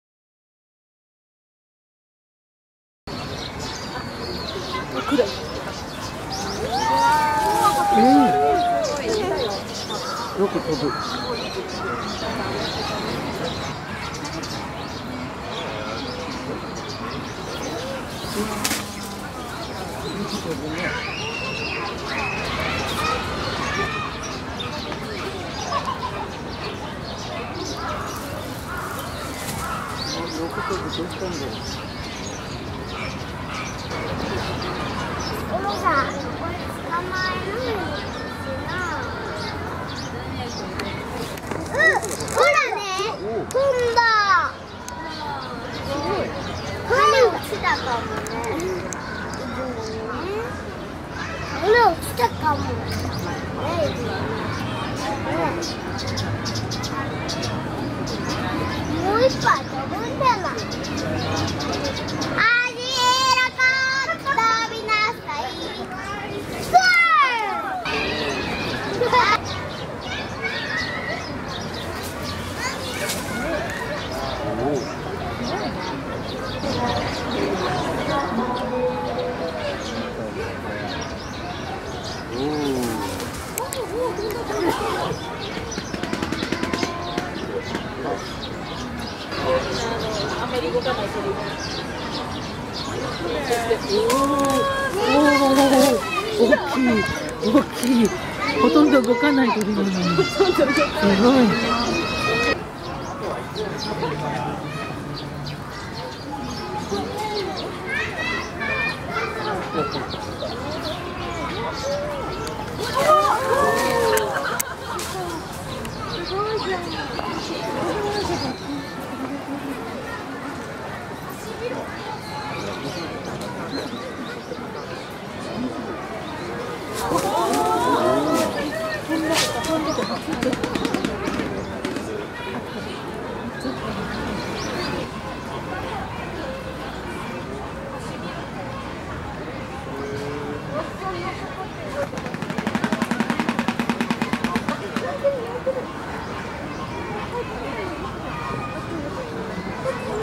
shoebill、ハシビロコウの飛翔
撮影日：2015/11/6　この日のシュシュ・ルタンガは午後二時頃から飛びまくっていました。